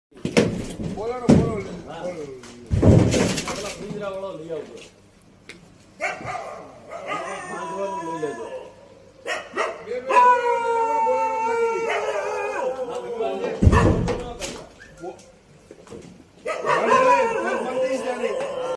Deer in ATM: कुत्तों से बचने के लिए ATM में घुसा हिरण, Video हुआ Viral
A deer was stuck in an ATM vestibule in Gujarat after dogs reportedly chased it. In a video, now viral on internet, the deer can be seen trying to find a way out of the ATM but in vain. It even collided against the wall in an attempt to break free as he realised the main glass gate in front of him was crowded by people armed with phone cameras and barking dogs.

The incident occurred in Amreli Dhari. It is not clear when the incident took place but it looks like the video was shot at night.

The panicking deer jumped and got restless in the kiosk, unable to get out. It was also scared of the barking dogs outside.

Later, forest officials came to the rescue of the helpless animal and released it back into the forest.